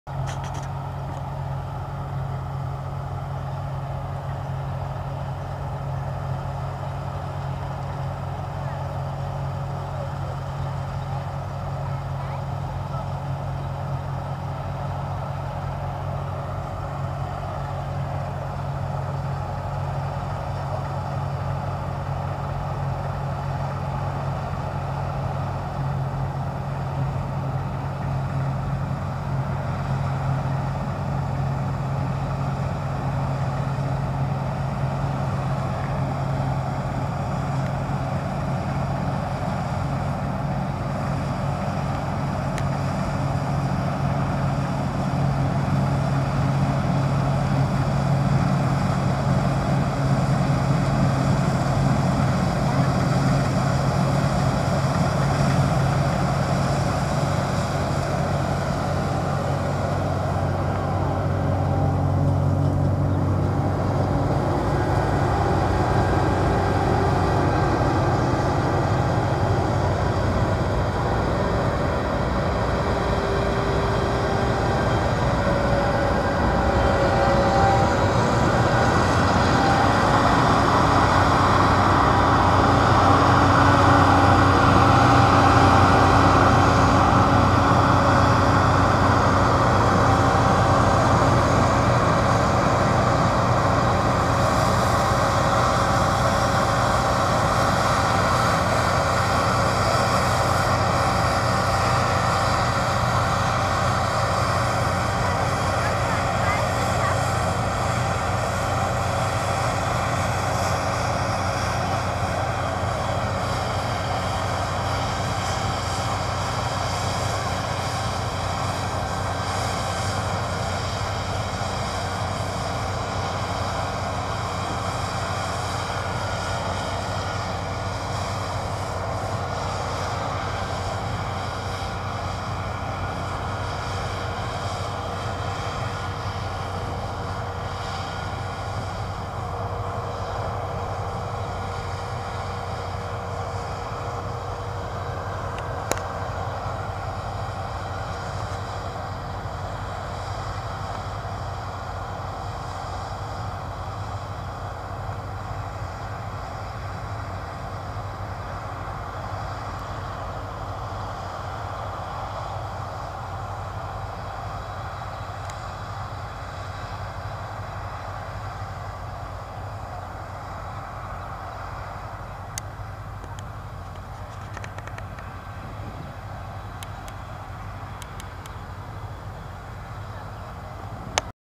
Ernte 2011
Wintergerste Ernte 2011 bei Agro Bördegrün in der Magdeburger Börde. Mit Mähdrescher Massey Ferguson Cerea 7278 beim Dreschen.